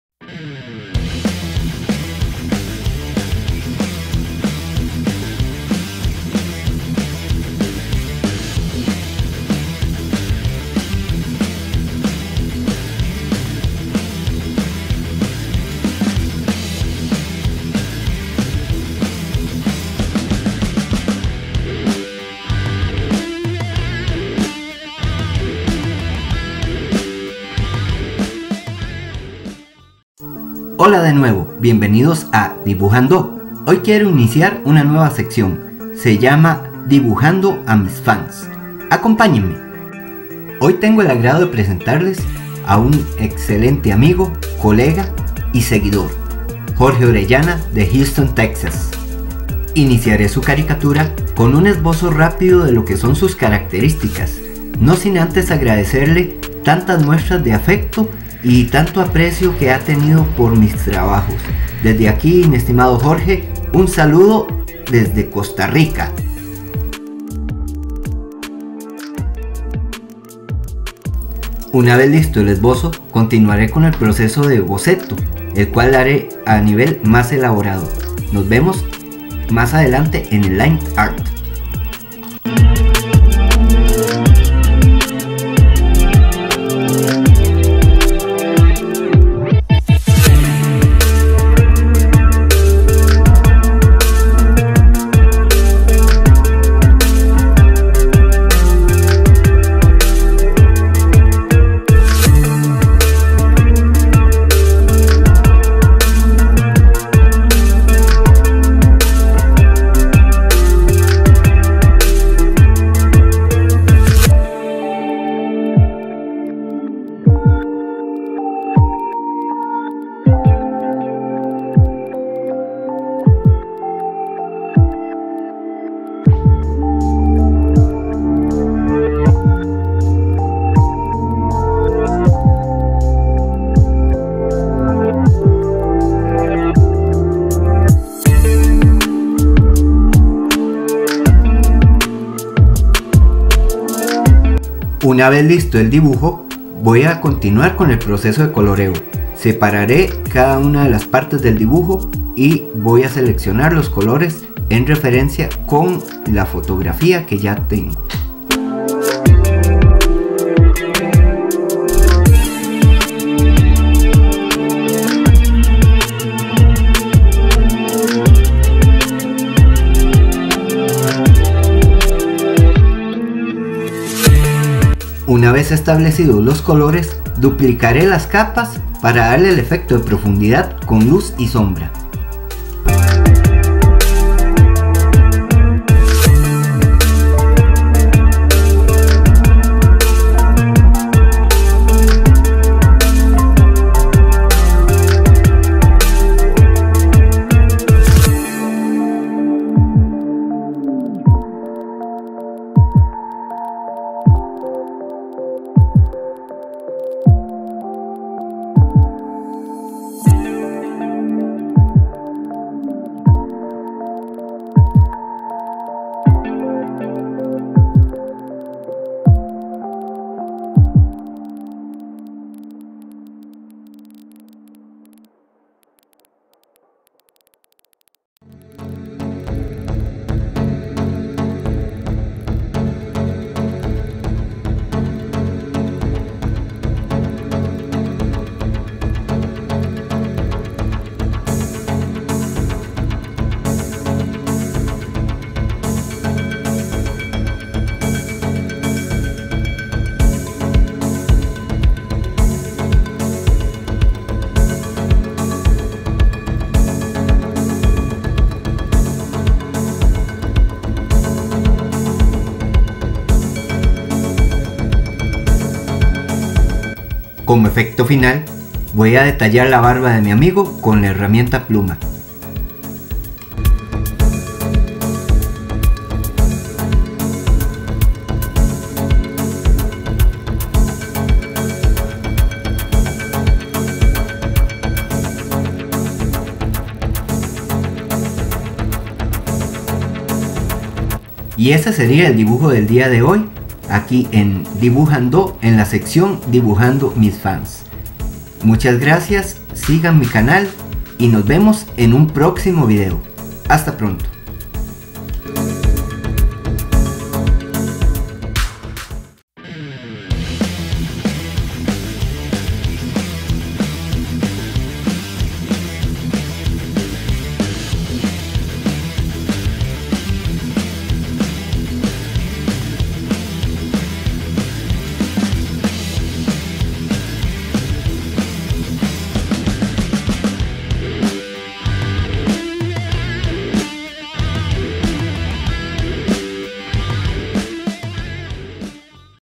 Dibujan Do a mis fans.. Jorge Orellana
Hoy inicio una sección semanal dentro de mi canal como forma de agradecimiento a mis seguidores tanto en Youtube  como en Facebook e Instagram.. Hoy mi gran amigo y seguidor Jorge Orellana de Houston Texas